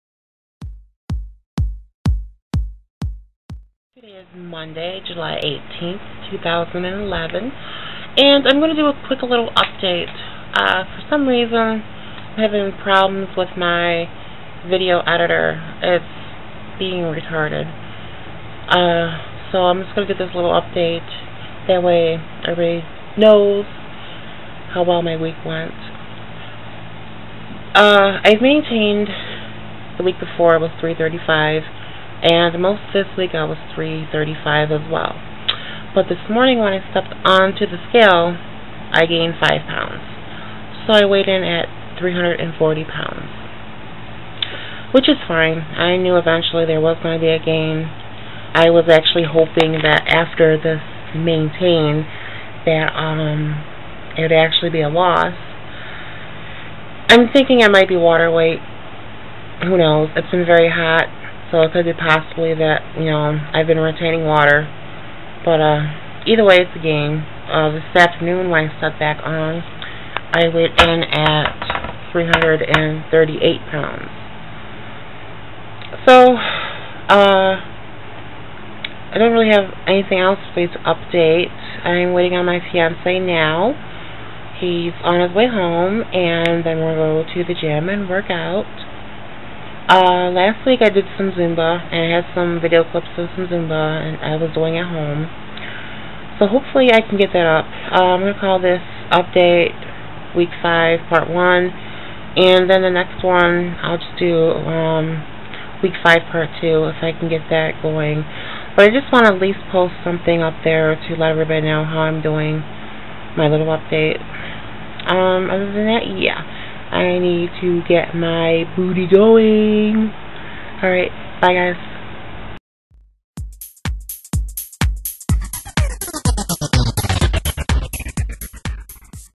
Week 5 quick update part 1
Just a quick update for week 5.Finally got my video editor working late last night.I wanted to post this,so I can at least update my week.It was a gain week :( 5 lbs could be water weight,but who knows.Its been so hot and my water intake has been insane.Either way it is still a gain.I will post the second half as soon as I can.As always hanks for watching !!!